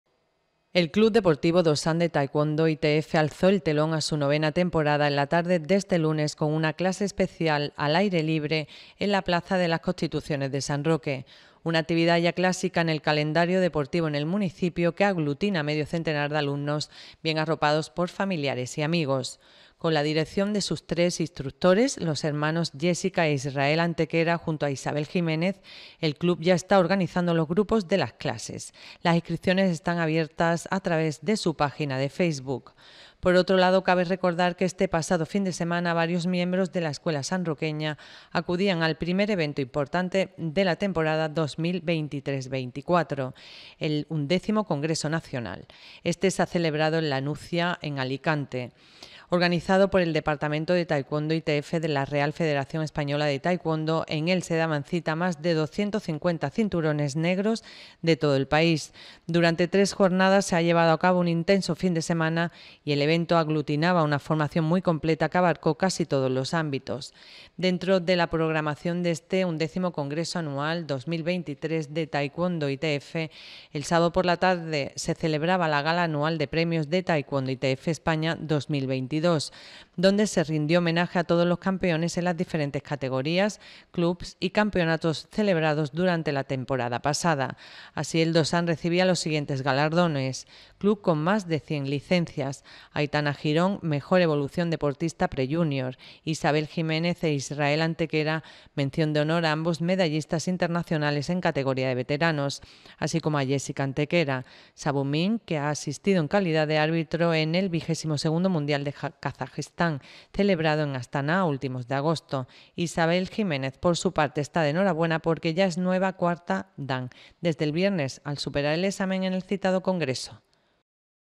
NOTICIA INICIO TEMPORADA TAEKWONDO ITF DO SAN 5 SEPTIEMBRE 2023
La Plaza de las Constituciones de San Roque se convirtió en la tarde de este lunes, 4 de septiembre, en el escenario de apertura del nuevo curso del Do-San de Taekwon-Do ITF como escuela deportiva de base.